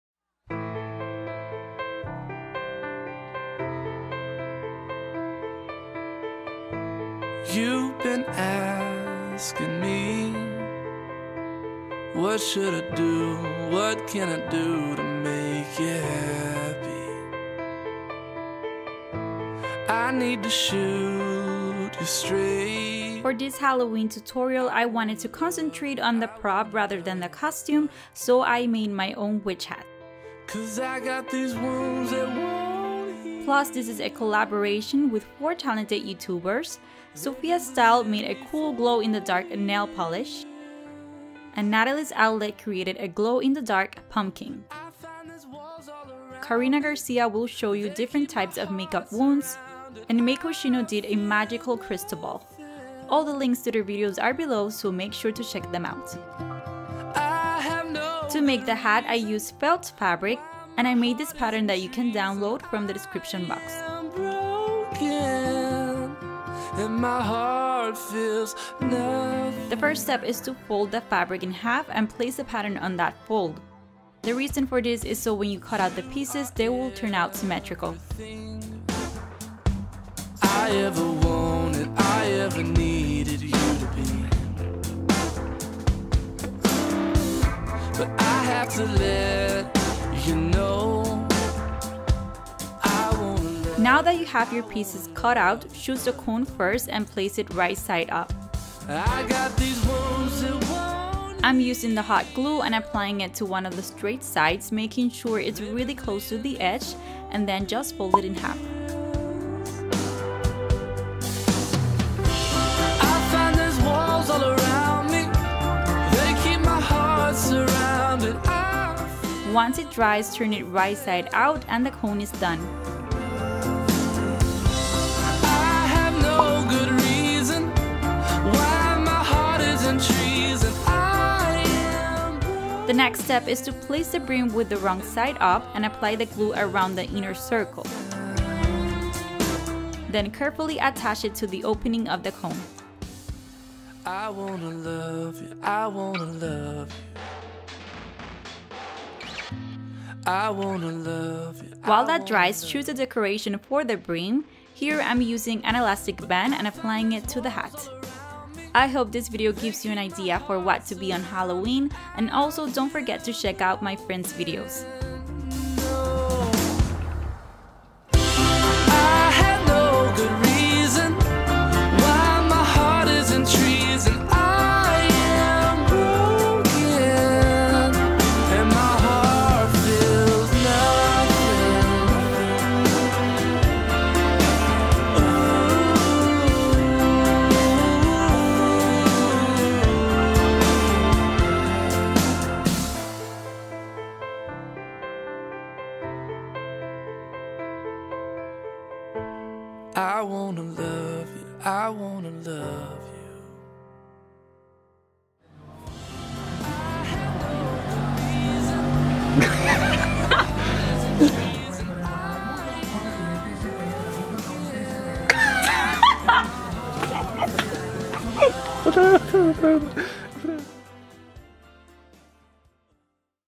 👻 DIY HALLOWEEN COSTUME | How to Make a DIY Witch Hat for a Halloween! ✨Alejandra's Styles
DIY Witch Hat Costume for Halloween! Learn how to make a DIY Witch Hat Costume for Halloween!  To make this DIY WItch Hat Costume you will need felt fabric and hot glue.  I created a pattern for the witch hat so it's easier for you to re-create.  Let me know if you are going to try out this Halloween idea!

Let's talk!